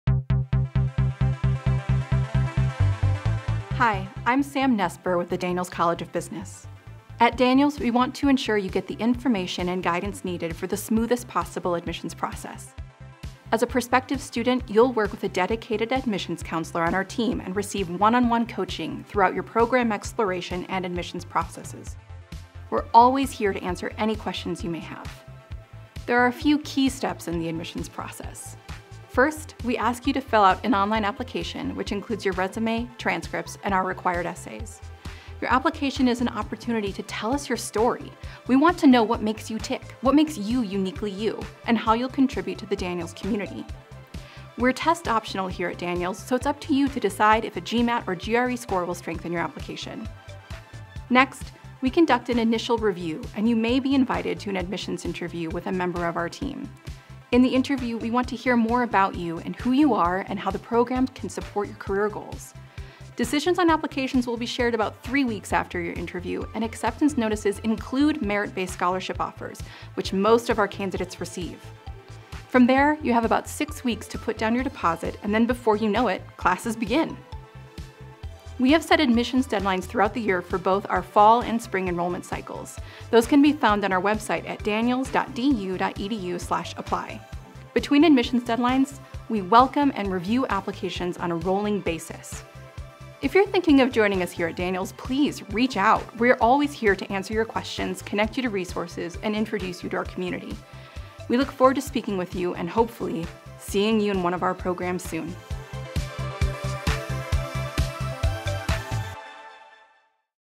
Daniels College of Business - The Graduate Admissions Process
Director of Graduate Admissions Sam Nesper shares information about the admissions process for the master's programs here at the Daniels College of Business.